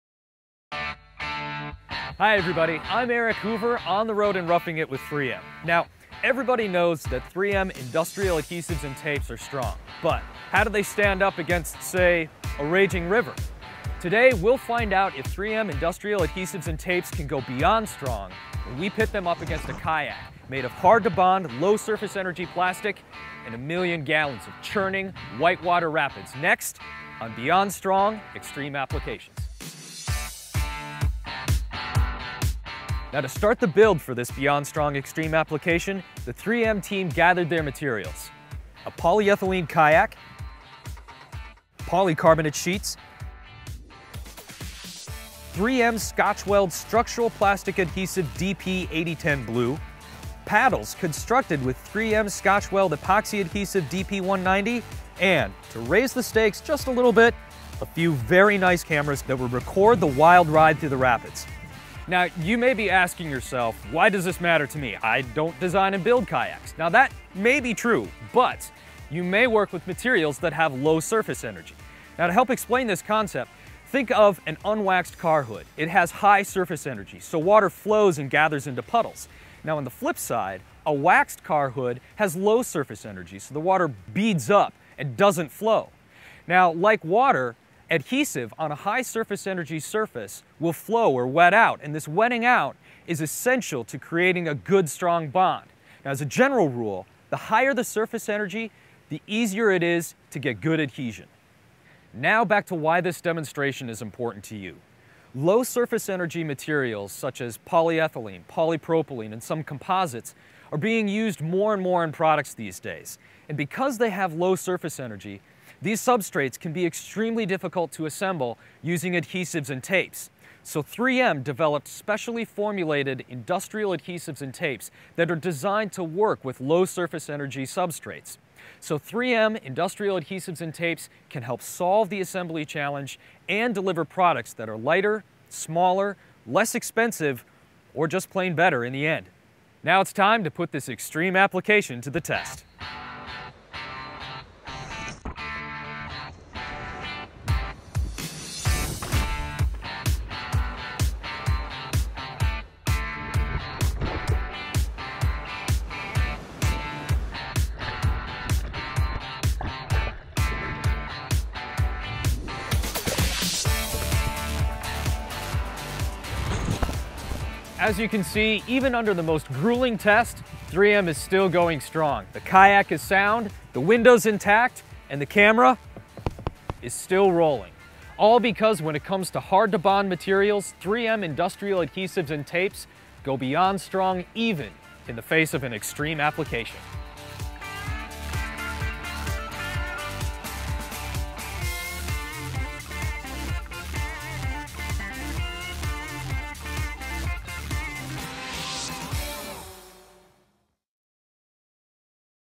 3M Assembly Solutions Extreme Applications: Hard to Bond Materials with Adhesives and Tapes
3M industrial adhesives and tapes are strong. But how do they stand up to say, a raging river?

Find out if 3M adhesives and tapes can go beyond strong when we pit them against a kayak made of hard-to-bond, low-surface-energy plastic – and millions of gallons of churning white-water rapids.

Why does this demonstration matter to you? Low surface energy materials like polypropylene, polyethylene and some composites are being used to build more and more products. 3M has developed specially formulated adhesives and tapes, designed to overcome the challenges of low surface energy substrates

Visit 3M.com/assembysolutions for more information and to see how 3M Industrial Adhesives and Tapes can solve your assembly challenge.